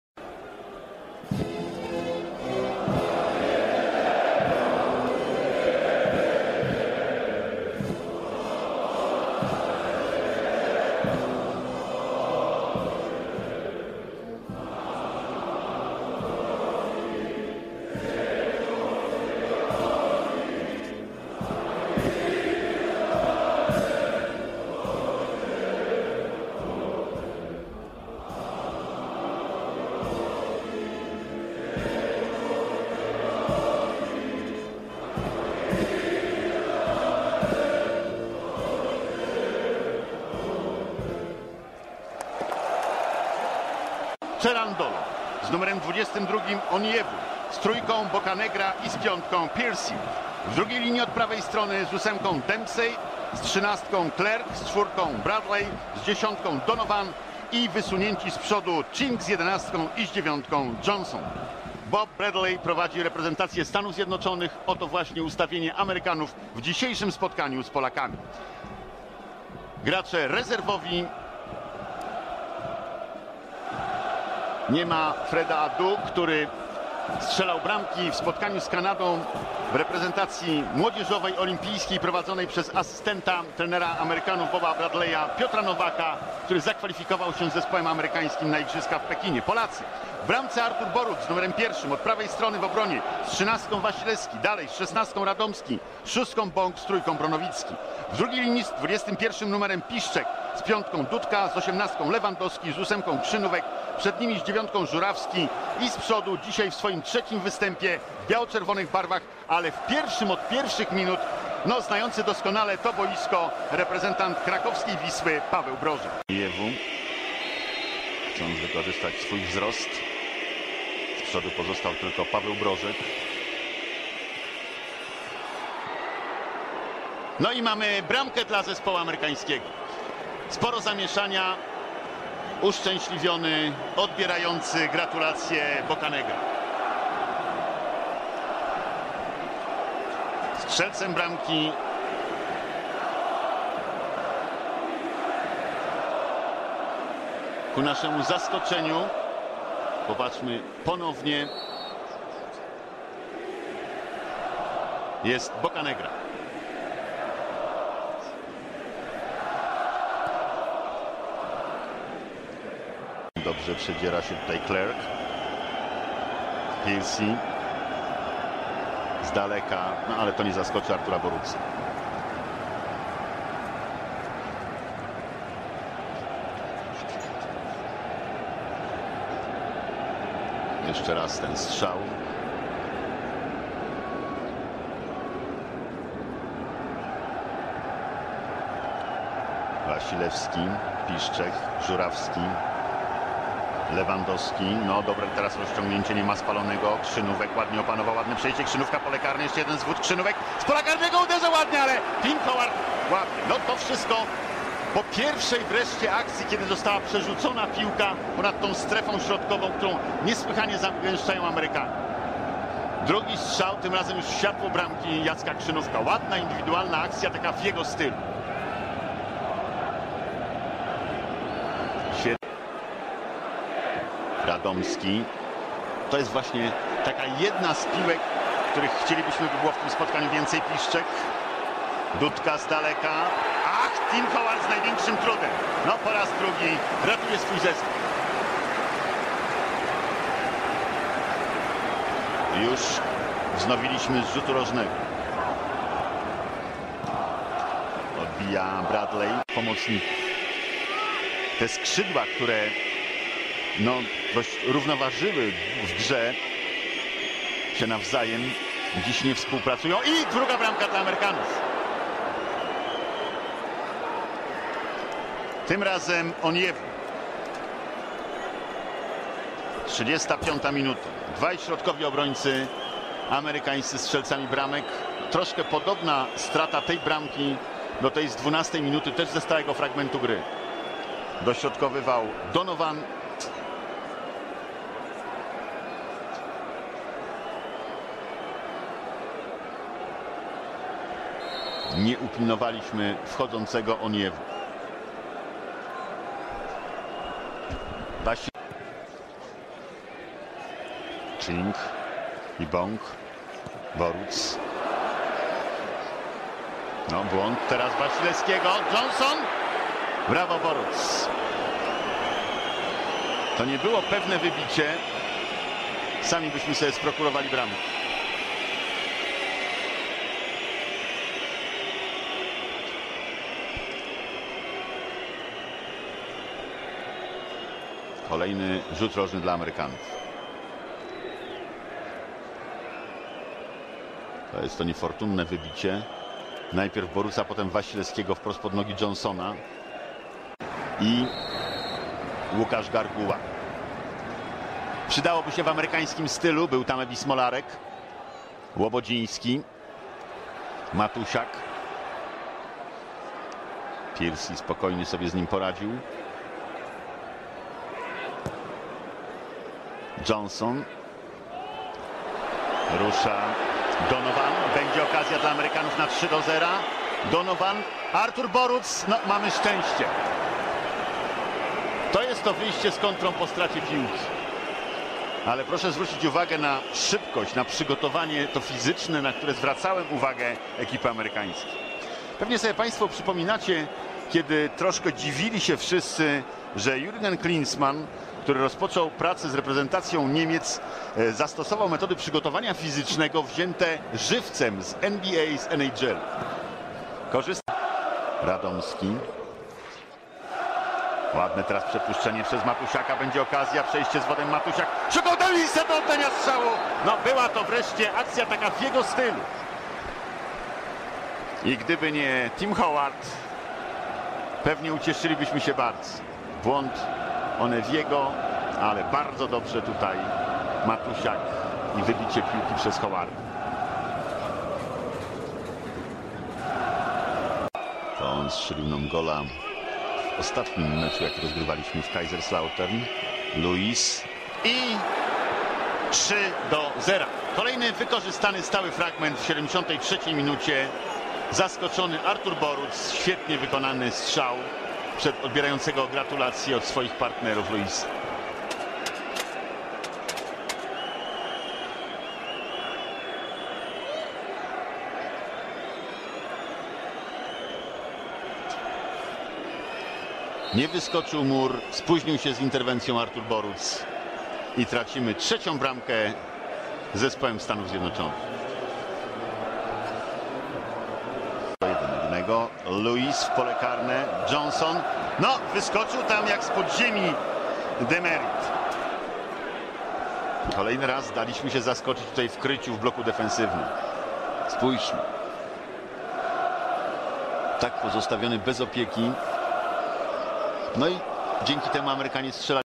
[693] Polska v USA [26/03/2008] Poland v United States
2008 [693] Polska v USA [0-3] Poland v United States
Mecz towarzyski, 26 marca 2008, Kraków, Stadion Wisły im. Henryka Reymana, widzów: 21 000, Sędzia: Anders Hermansen (Dania)

Bramki: 0-1 Carlos Bocanegra 12', 0-2 Oguchi Onyewu 35', 0-3 Eddy Lewis 73'

Polska: Artur Boruc - Marcin Wasilewski, Jacek Bąk, Arkadiusz Radomski (63' Michał Goliński), Grzegorz Bronowicki - Łukasz Piszczek (46' Wojciech Łobodziński), Dariusz Dudka, Mariusz Lewandowski, Jacek Krzynówek (46' Euzebiusz Smolarek) - Maciej Żurawski (K) (46' Łukasz Garguła), Paweł Brożek (46' Radosław Matusiak), Trener: Leo Beenhakker

USA: Timothy M. Howard - Steven Cherundolo (73' Jonathan M.P. Spector), Oguchialu J. Onyewu (63' Jay M. DeMerit), Carlos Bocanegra, Heath Pearce (85' Benny Feilhaber) - Clinton D. Dempsey, Michael Bradley, Ricardo A. Clark, Landon T. Donovan (64' Edward J. Lewis) - Brian Ching (63' Joshua D. Wolff), Edward Johnson